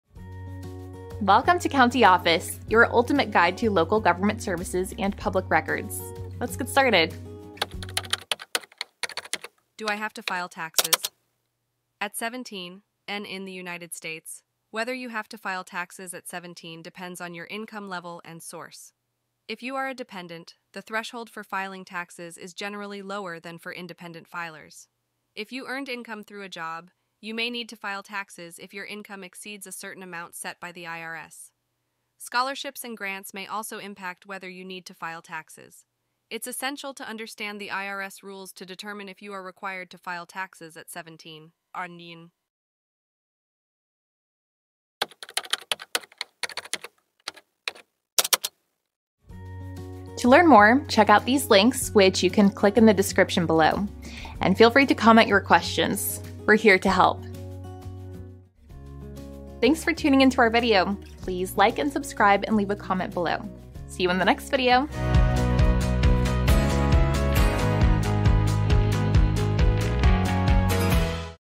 Do I Have To File Taxes At 17? - CountyOffice.org
Do I Have To File Taxes At 17? Have you ever questioned whether you need to file taxes at 17? In this enlightening video, we delve into the intricacies of tax filing requirements for individuals at 17 years old in the United States. Understanding the rules set by the IRS regarding income thresholds, dependency status, and sources of income is crucial in determining your tax filing obligations. Scholarships, grants, and earned income all play a role in whether you are mandated to file taxes. Stay informed and learn more about your tax responsibilities at 17 by watching this video.